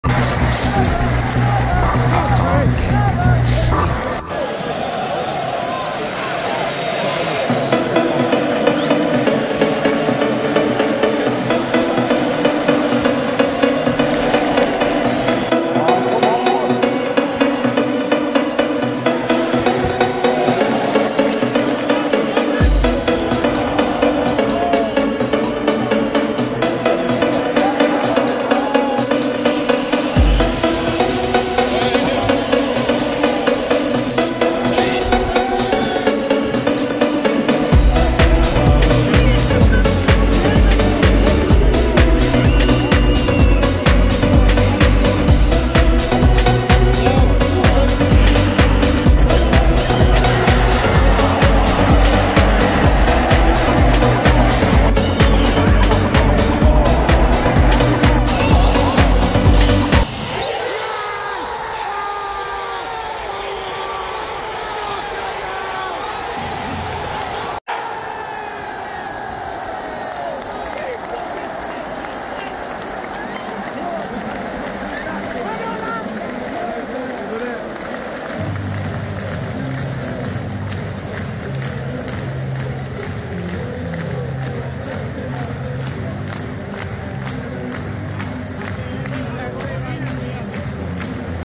Forest Manchester City what a game !
Forest get a great point with a draw against Manchester City what a game and atmosphere nffc football forest nottinghamforest premierleague nottingham mufcclips #nottinghamforest